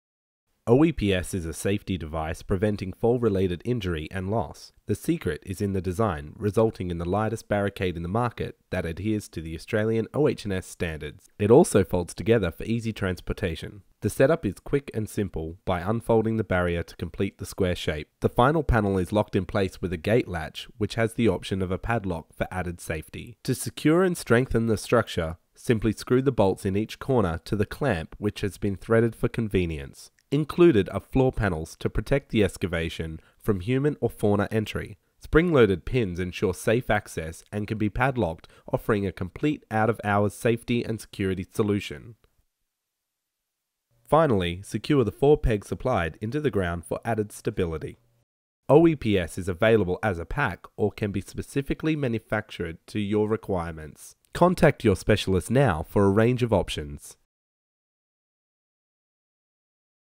OEPS Barriers | Open Excavation Protection Systems | Fall Protection
OEPS Barriers provide Open Excavation Protection Systems for work site protection of open holes and trenches.

OEPS Safety Barriers fall into two main categories:
Trench Protection Systems (TPS)
Hole Protection Systems

that are designed to prevent fall related injuries to team members, contractors, the public and fauna.

OEPS fall protection equipment is based on a patented floor lock engagement system. This ensures OEPS floor integrity is maintained whether the barrier gate is open or closed.